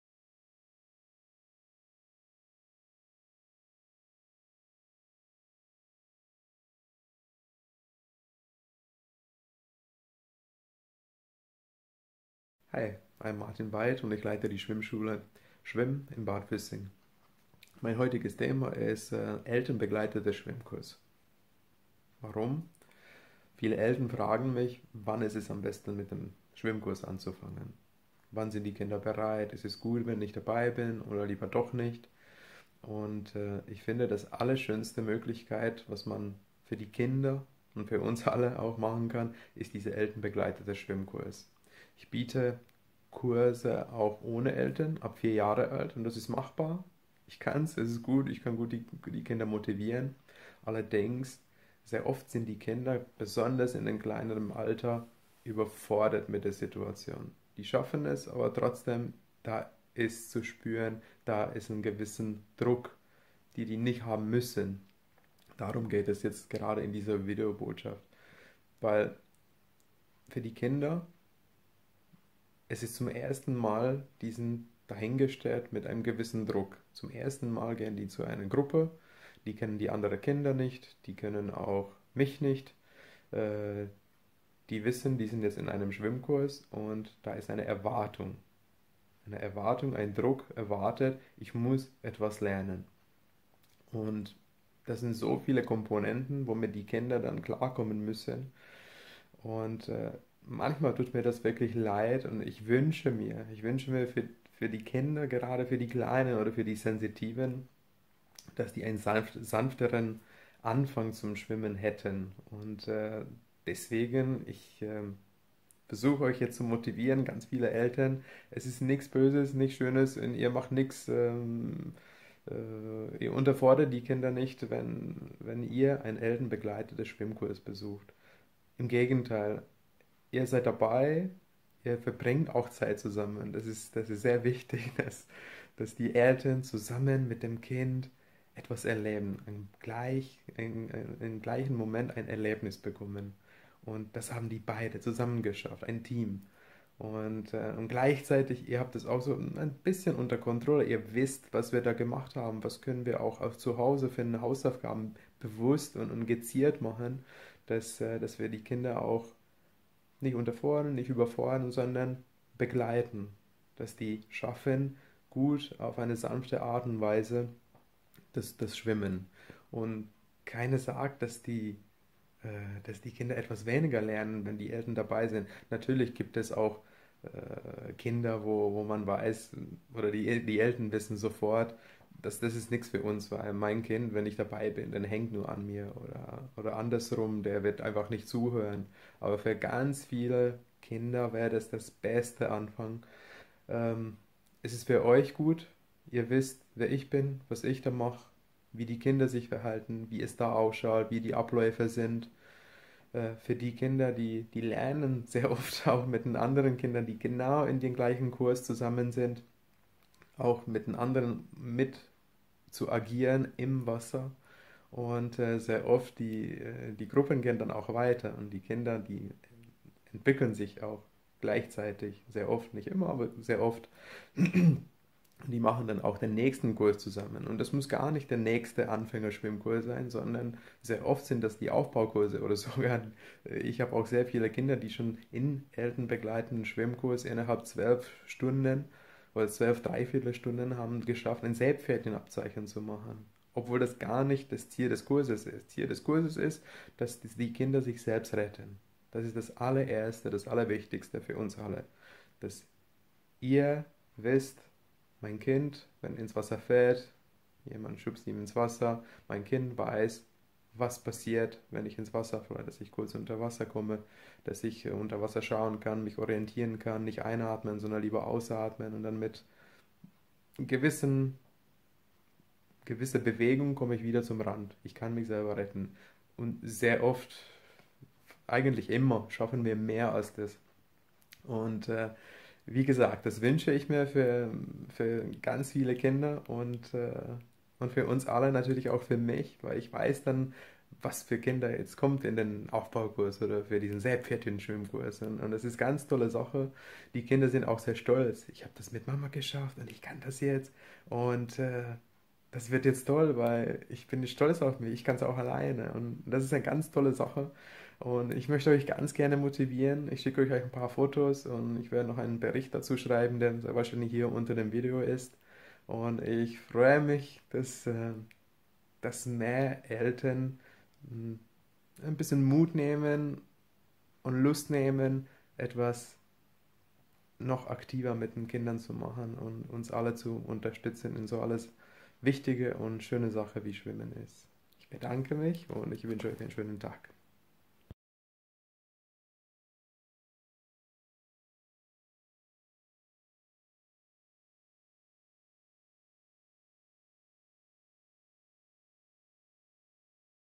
Elternbegleitete Schwimmkurs mit SchwiM-M - bei Martin White
für Kinder ab 4 Jahre.
Besonders für Kinder die sensitiv sind und kein Druck brauchen.
Es ist der sanfteste Anfang und Vorbereitung, nicht nur für die Schwimmkurse, sondern auch für die Schule selbst. Dem Kinder ist es Bewusst, dass es gerade in einer Schwimm SCHULE ist. Gemeinsam mit den Eltern, machen die Kinder den erste Schritt zu Konzentration, Lernen wollen, Mut sammeln und sich Stück für Stück von den Eltern trennen und evtl. der nächste Kurs schon alleine besuchen und meistern.
In diesem Kurs geht es um viele Sachen, am Wichtigsten ist es,…
• dass wir Konsequent dem Kindern zeigen, dass Konzentration und gewisse Disziplin auch zur Spaß und letztendlich zur Erfolg führt
• dass Wasser und Bewegung in diesem Element etwas wunderbares ist
• dass man dem Element Wasser vertrauen kann und was alles da drinnen machbar ist
• dass wir die Kinder begleiten zu selbständigen Wesen sind, die sich darüber freuen und auf sich selbst stolz sind
• dass die Kinder die Selbstrettung schaffen und wissen was zu tun ist in Gefahr